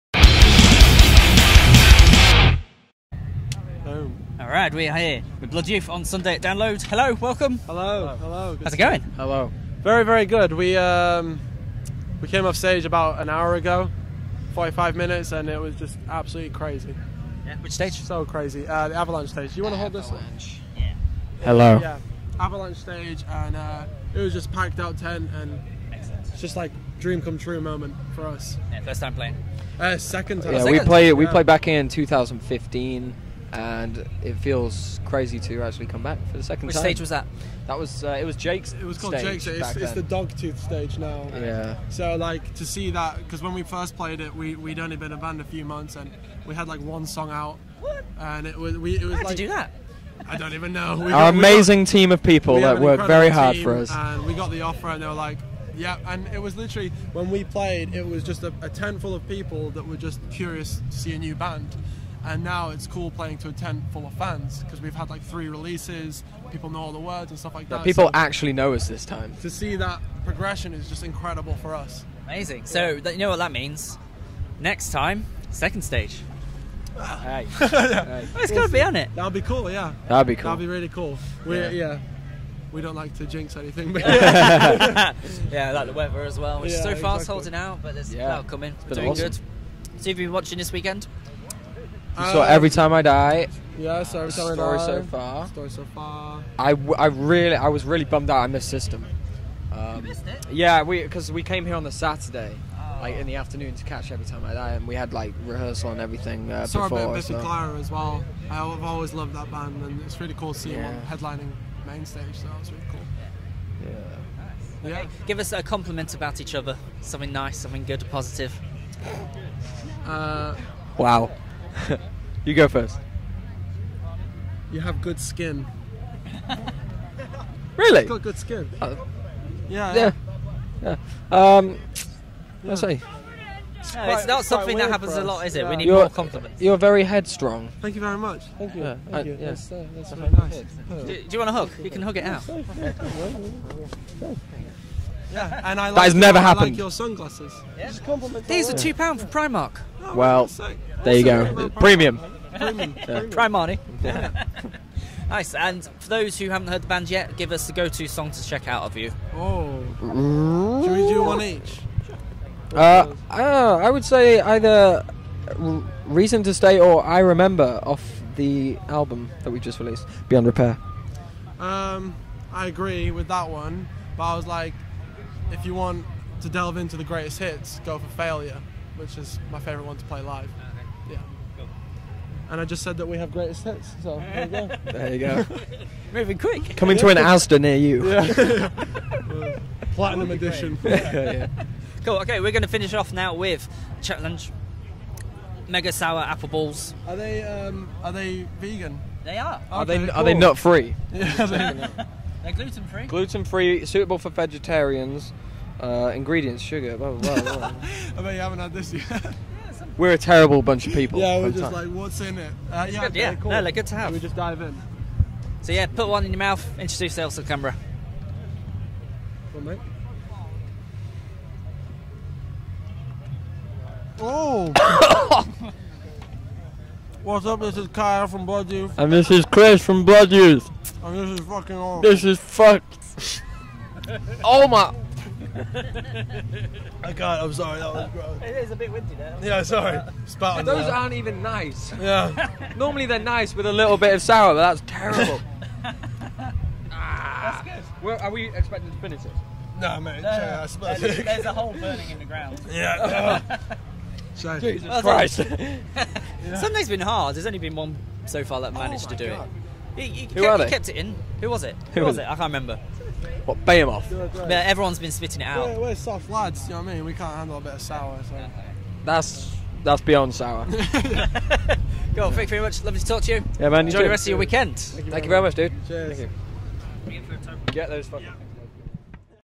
Blood Youth interview Download 2017 (TotalRock)
Xander meets Blood Youth and chat playing Download!